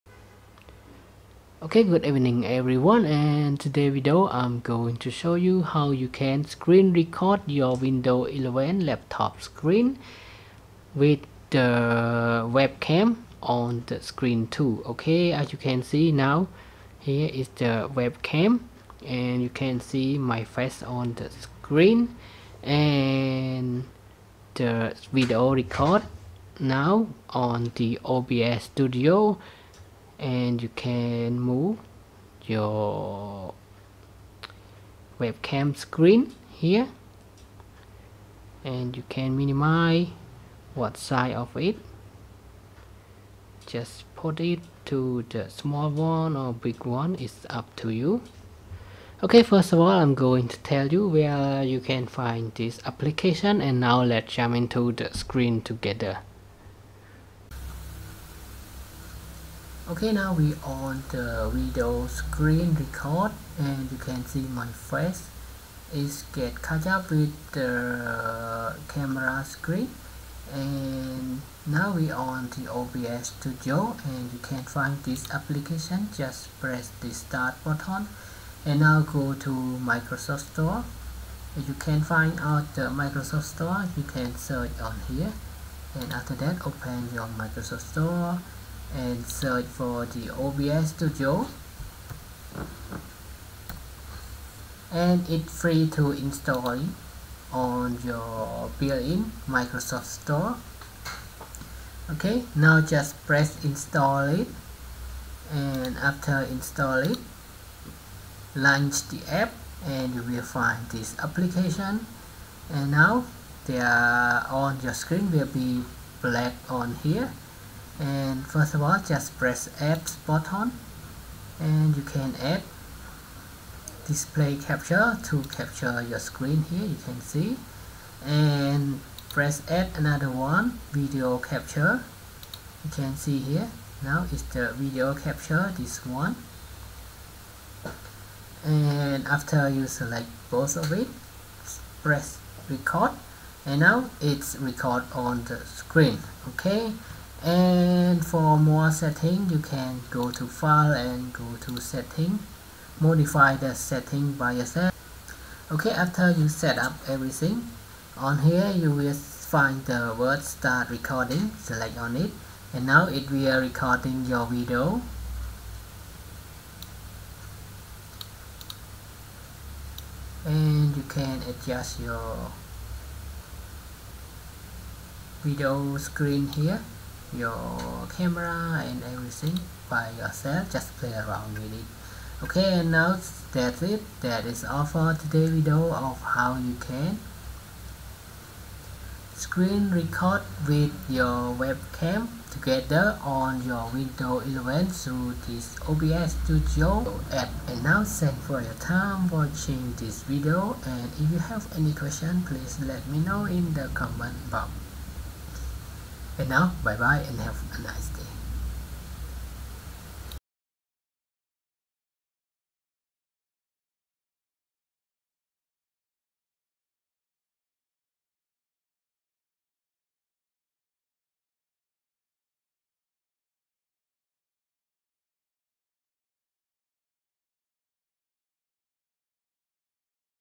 How to Video Screen Record with Webcam on Windows 11
In today's video, I am going to show you how to video screen record with webcam camera on Windows 11.

First of all you need to make sure that your laptop has webcam or if not you can connect to the additional webcam as I shown in the video. After that download the OBS studio in Microsoft store. Open it and follow the instruction in the video and you good to go.

How to Video Screen Record on Windows 11